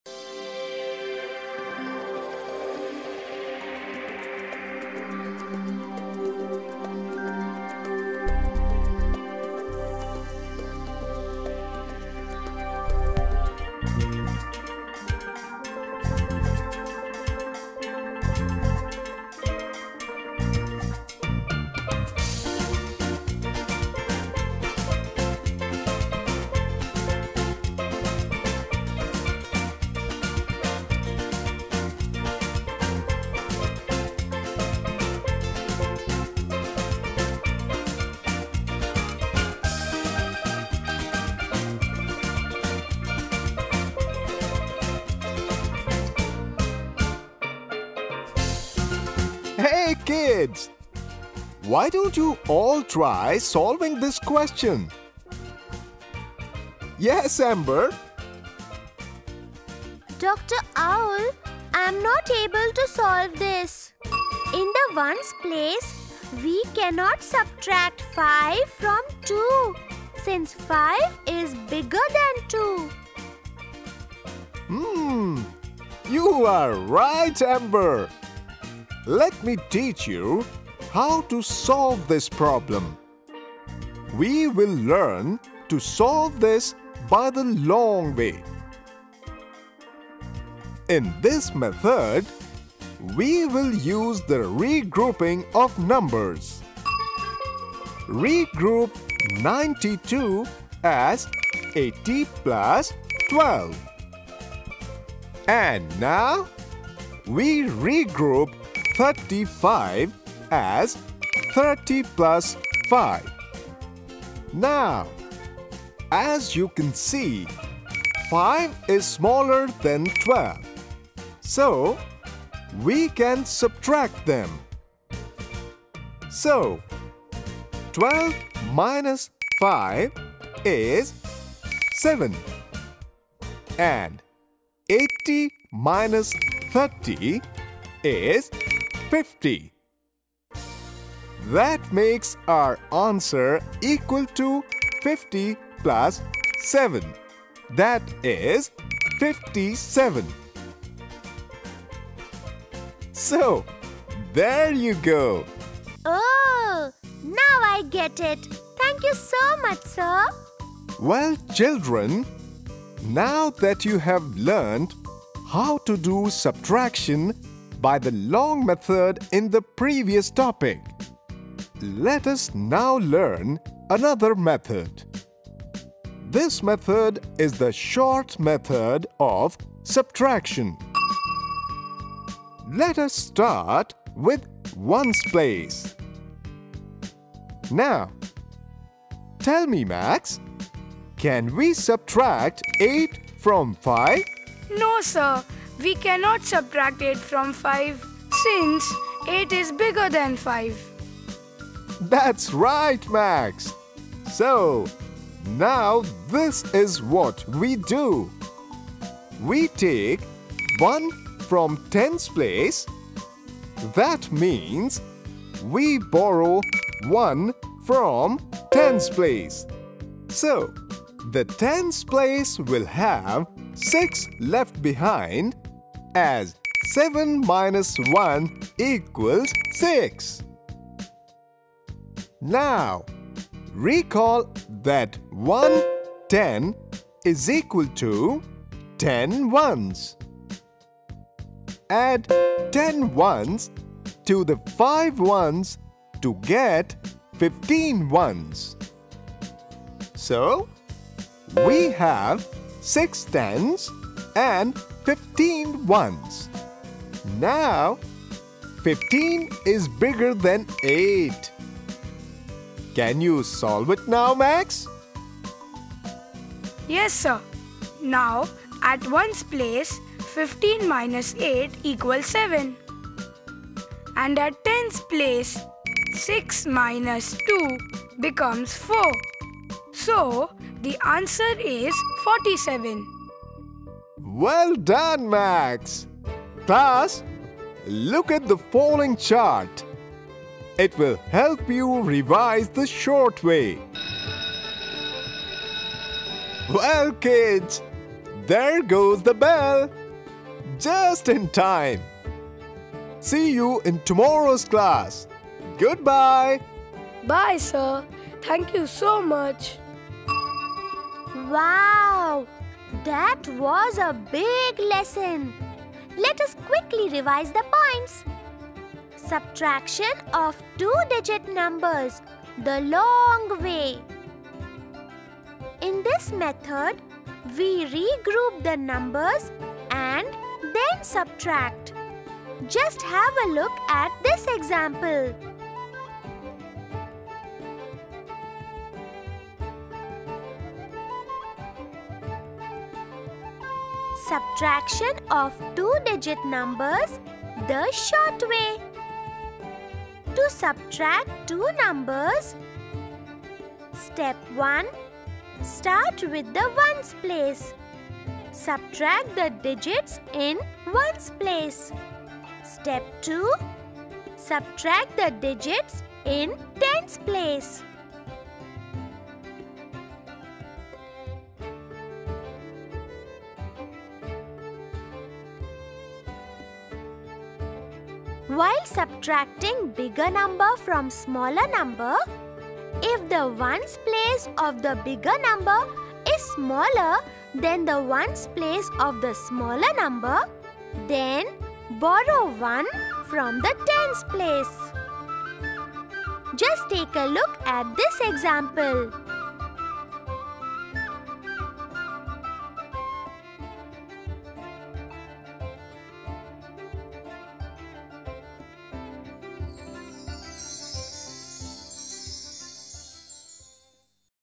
Learn Subtraction of 2 Digit Numbers by Long and Short Methods
Subtraction is an important part of mathematics. We start learning subtraction by subtract a digit from other. In this Video Dr. Owl teaches Amber and students how to subtract by long and short methods.
He teaches them in a very interesting way which interests you and till the end you realize that you are now aware all about subtraction.